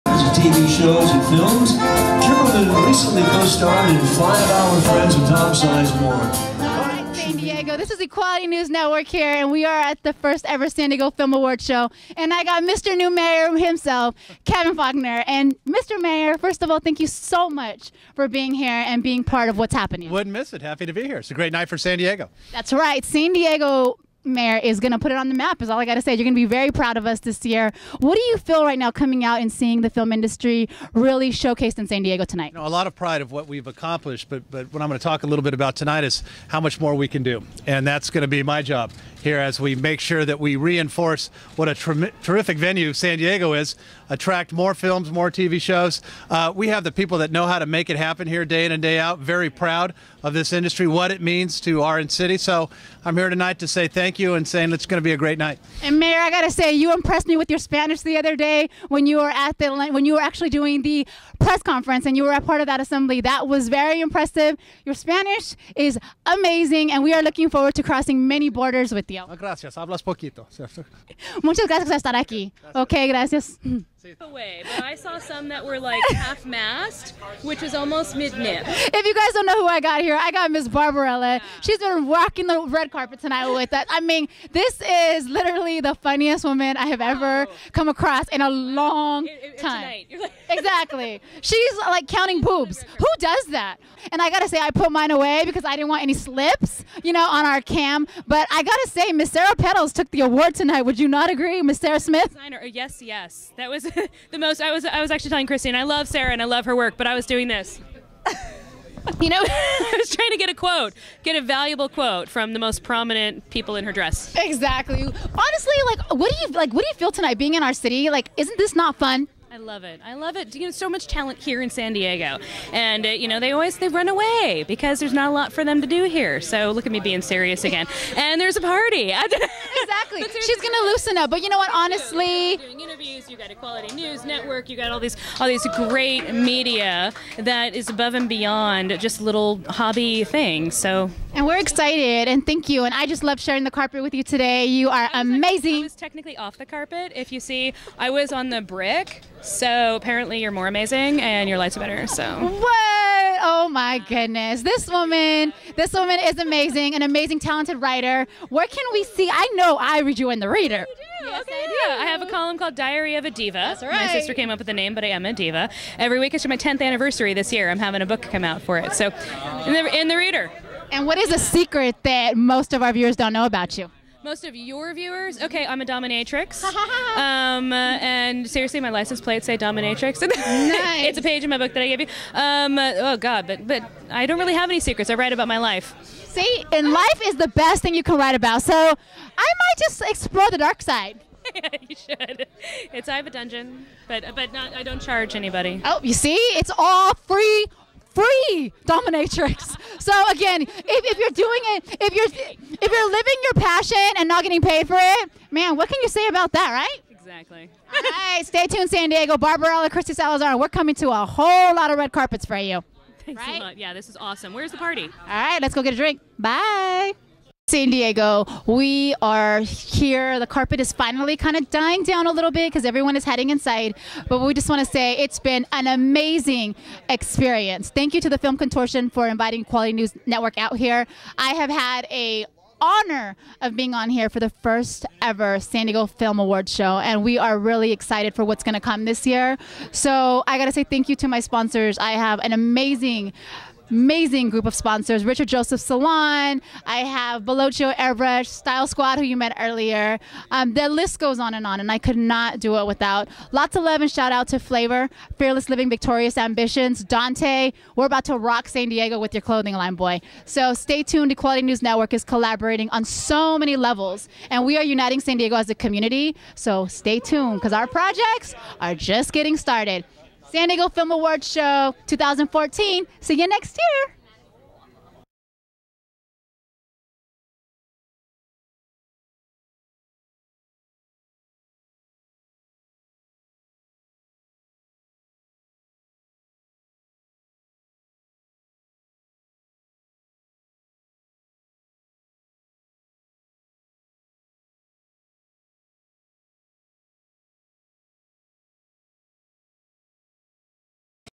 San Diego Film Awards 2014 Mayor Kevin Faulconer, Barbarella
FIRST EVER Film Awards for San Diego filmmakers, cast and crew. The Film Consortium San Diego has been active in the community since late 2012 and has generated excitement among San Diego filmmakers, cast and crew by actively connecting the community, creating new opportunities for filmmakers to work together, building an infrastructure for local film production and offering educational opportunities.

"This community is growing fast and I can see San Diego back on the map as a production destination in the next few years," said Film Consortium San Diego founder and president, Jodi Cilley.  "As a true indicator of the sleeping giant we have here, we have collected over 200 locally produced film projects and are finally ready to celebrate and award the best of the best."